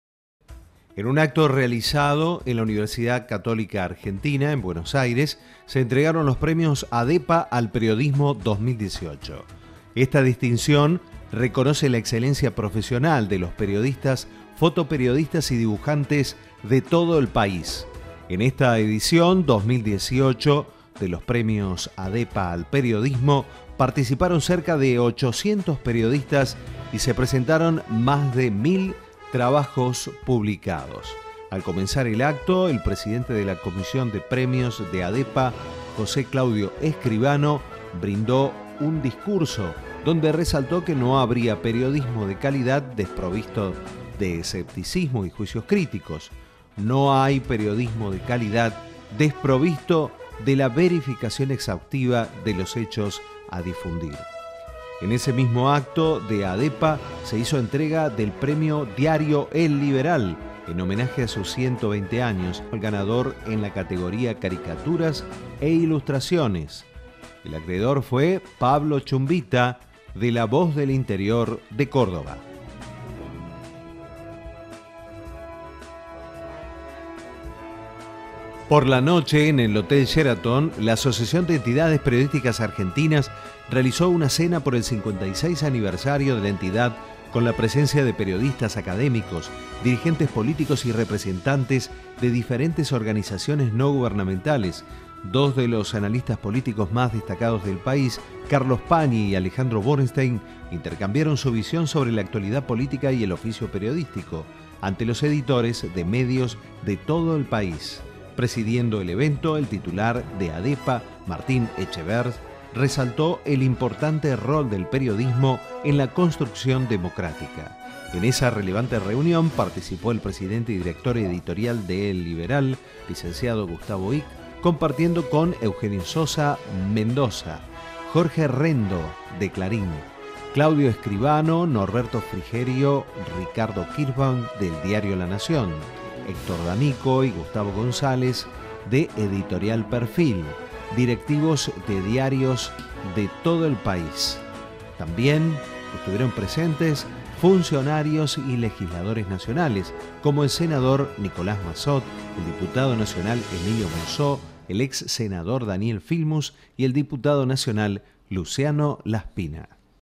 Entregaron los Premios Adepa al Periodismo 2018
El Premio Adepa/Diario EL LIBERAL (Santiago del Estero), para la categoría Caricaturas e Ilustraciones, el primer premio lo obtuvo Pablo Fernando Chumbita (La Voz del Interior, Córdoba).